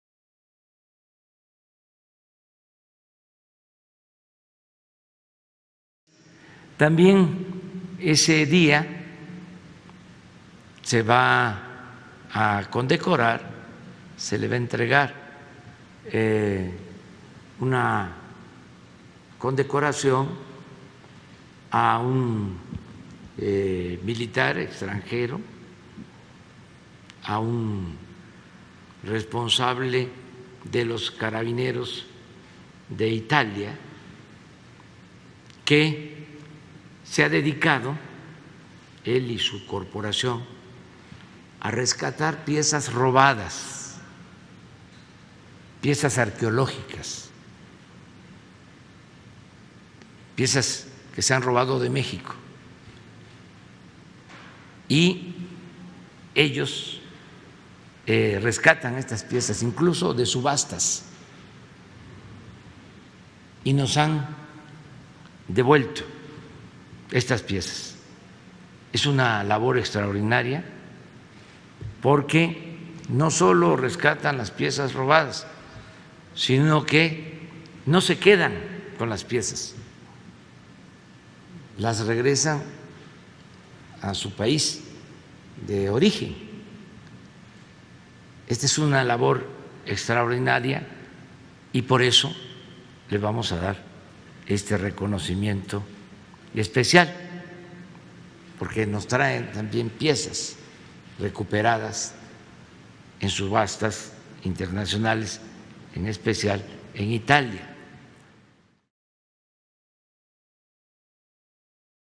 L'Aquila Azteca ai Carabinieri TPC / El Águila Azteca a los Carabineros de Italia
Il presidente del Messico Andrés Manuel López Obrador ha annunciato che domani, lunedì 27 settembre, insignirà il generale italiano Roberto Riccardi, capo del Comando Carabinieri per la Tutela del Patrimonio Culturale, dell'Ordine messicano dell'Aquila azteca, in riconoscimento al recupero di pezzi archeologici trafugati.
El presidente de México Andrés Manuel López Obrador informó que mañana, lunes 27 de septiembre, condecorará con la Orden Mexicana del Águila Azteca al general italiano Roberto Riccardi, jefe del Comando de Carabineros para la Tutela del Patrimonio Cultural, en reconocimiento por la recuperación de piezas arqueológicas robadas.